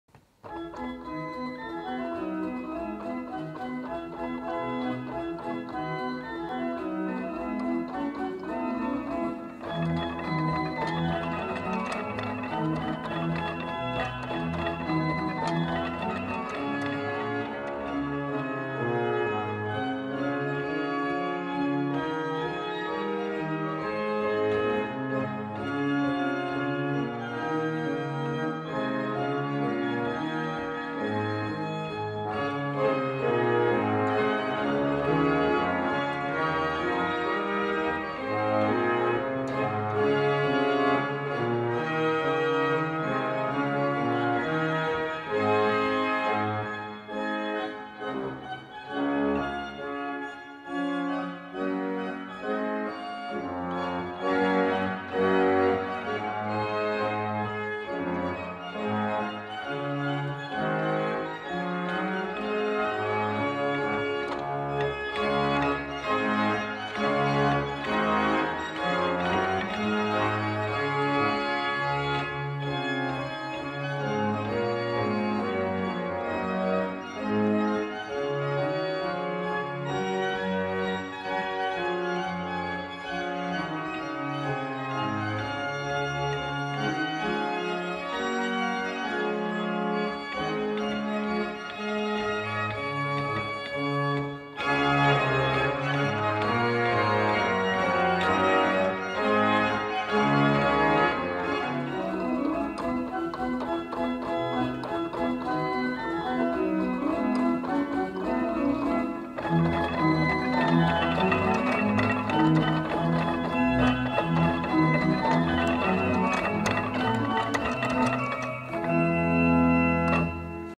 1065 Organist Ad Van de Wege Hooghuysorgel Maldegem 3-6-2012/FPAVDW
Dag van de Open Kerken 3 juni 2012.  Organist Ad Van de Wege speelt op Hooghuysorgel in de Sint-Barbarakerk te Maldegem.